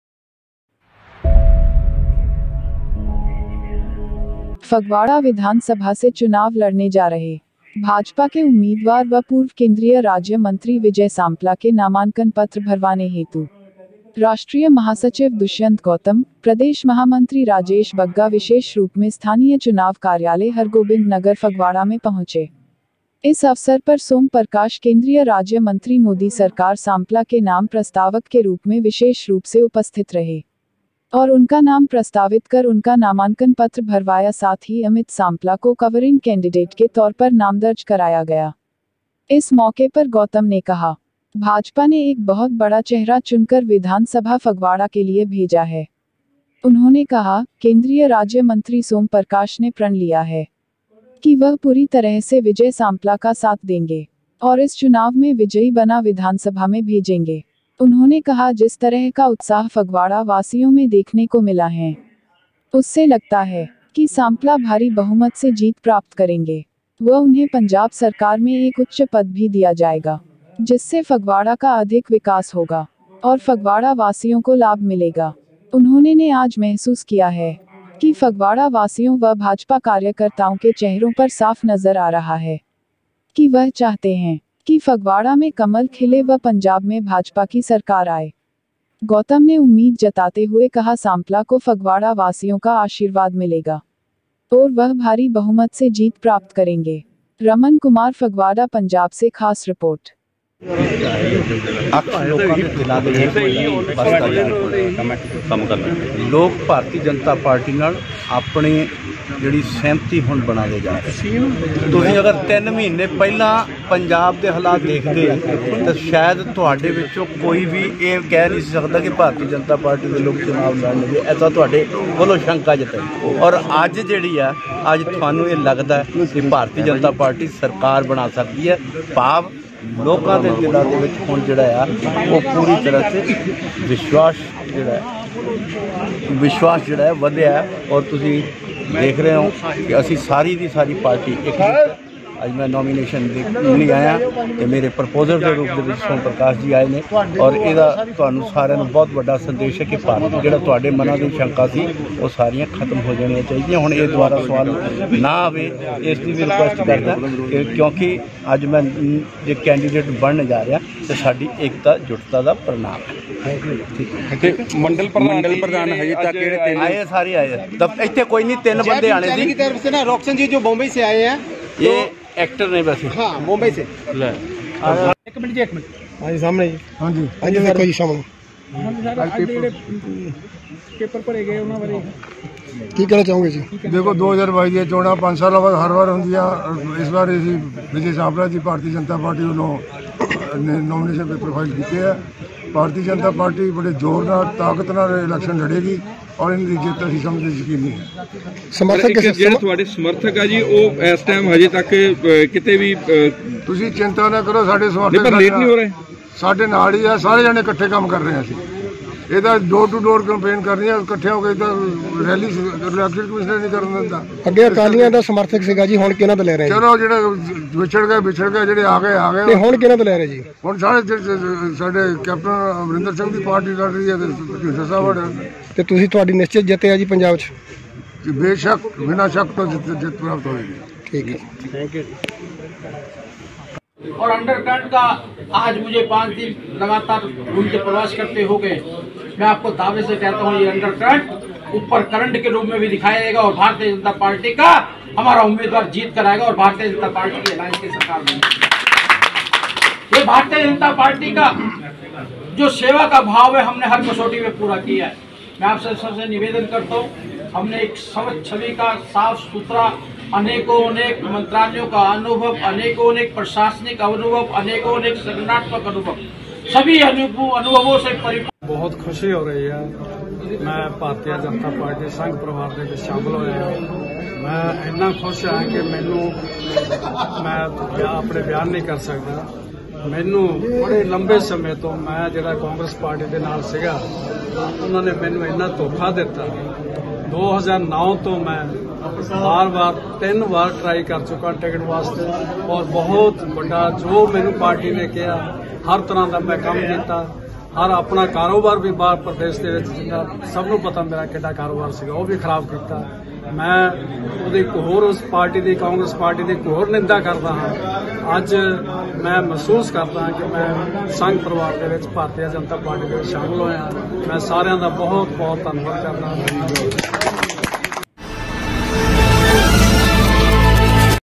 विजय सांपला ने दाखिल किया नामांकन, सोमप्रकाश बने प्रस्तावक। रमन कुमार / फगवाडा पंजाब / Raman kumar
फगवाड़ा विधान सभा से चुनाव लड़ने जा रहे भाजपा के उम्मीदवार व पूर्व केंद्रीय राज्य मंत्री श्री विजय सांपला जी के नामांकन पत्र भरवाने हेतु राष्ट्रीय महासचिव श्री दुष्यंत गौतम जी, प्रदेश महामंत्री श्री राजेश बग्गा जी विशेष रूप में स्थानीय चुनाव कार्यालय हरगोबिंद नगर फगवाड़ा में पहुंचे।